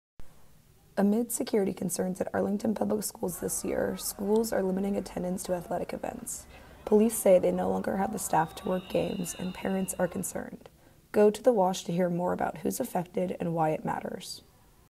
Arlington Public Schools limiting attendance at athletic events
High school football season in Arlington County will look a little different this year. Arlington County Public Schools announced a new policy limiting attendance at all school sporting events due to safety concerns.